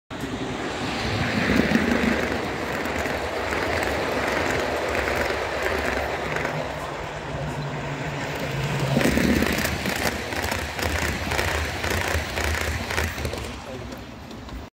Whitwick O Gauge Peak Day
A class 45 and 46 stretch their legs at Whitwick